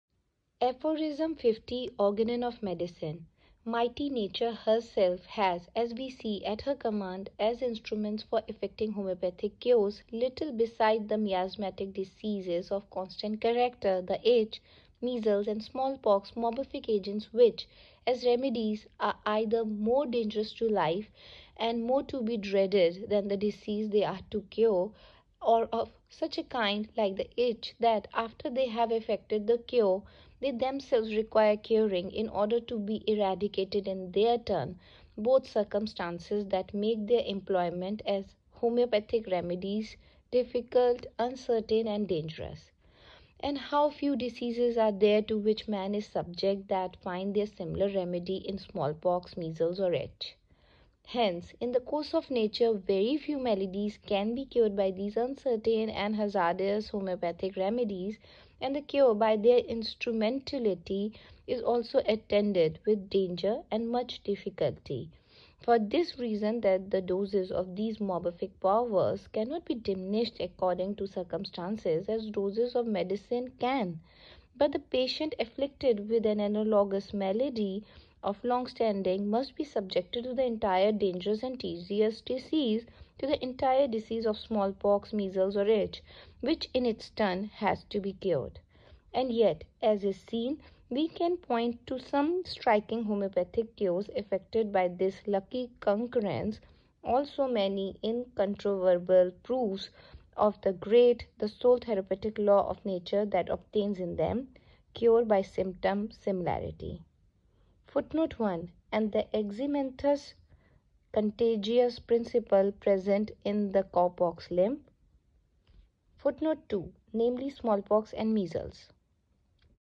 50 Aphorism- Difficult but the Only Therapeutic Law of Nature - Cure by symptom similarity !
Aphorism 50 - Organon of Medicine

Measles and smallpox as remedies are either more dangerous to life and more to be dreaded than the disease they are to cure, or of such a kind that after they have effected the cure,  they themselves require curing, in order to be eradicated in their turn, making their employment as homoeopathic remedy is difficult uncertain and dangerous

On the other hand man-made homoeopathic medicines powers can be diminished

Though by this lucky concurrence we can conclude the therapeutic law of Nature that is - Cure by symptom similarity !